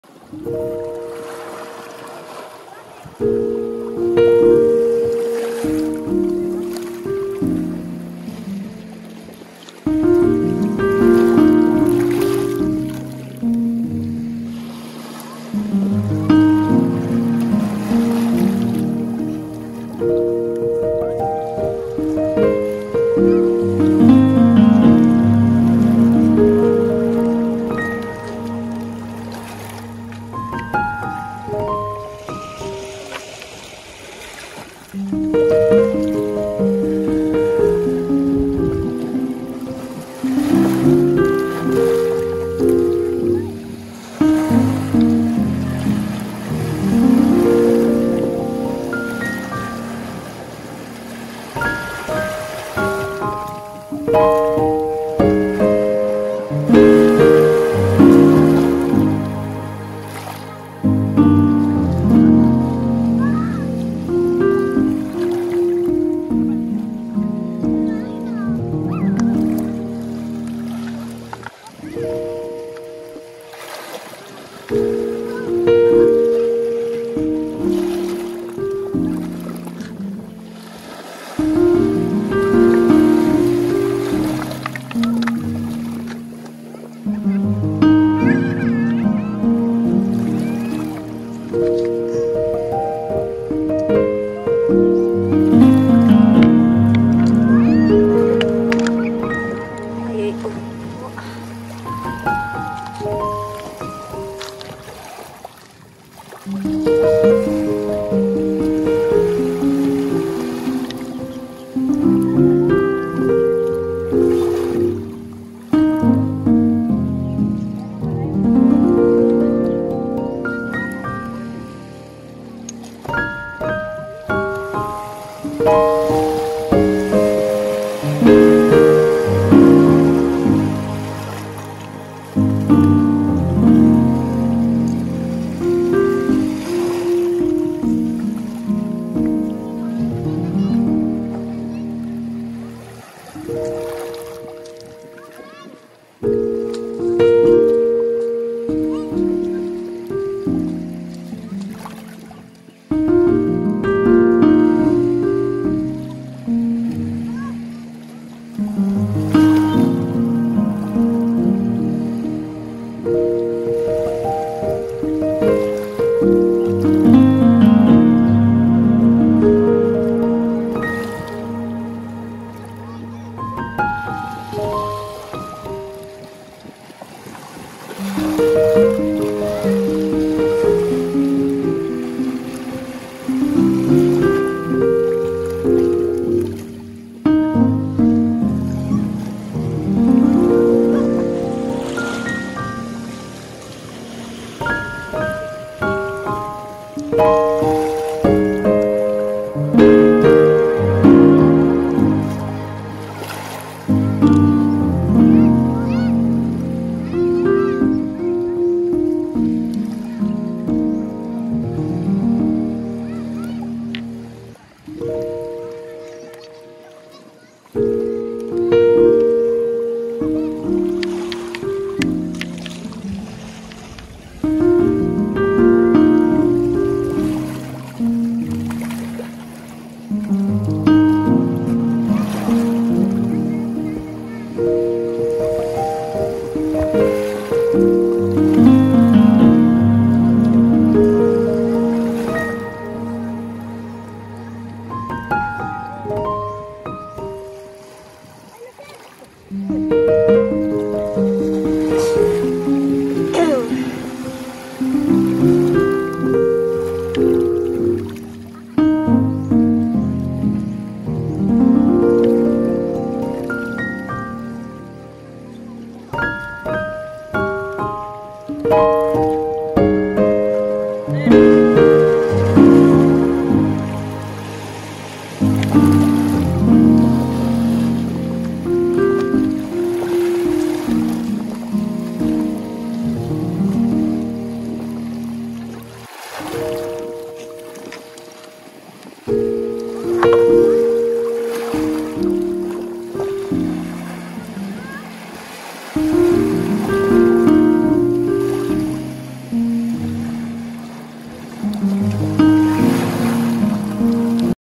RELAXING MUSIC IN THE BEACH| SOUND OF WAVES | BEACH VIEW IN CULIPAPA
Hello everyone! This is another relaxing video with relaxing music and view of the ocean. Hope you will enjoy this video.